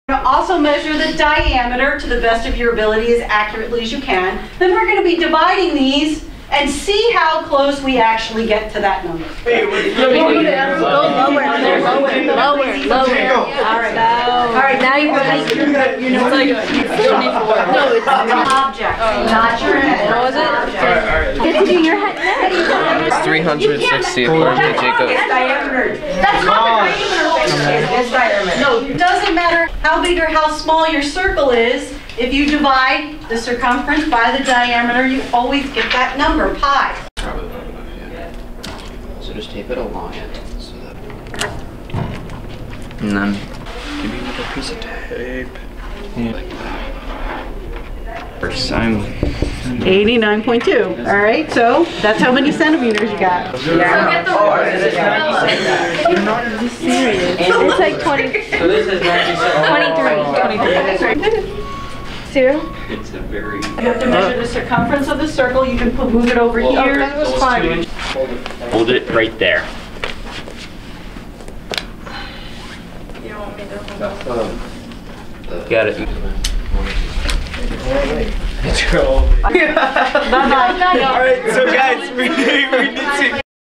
Pi Day Activities
Pi Day (Mar 14 or 3-14), is a day to celebrate the mathematical constant π. Ms. Bennett’s Algebra students marked the occasion by measuring the circumference and diameter of a variety of objects (including each other’s heads!) to see how closely the ratio came out to Pi.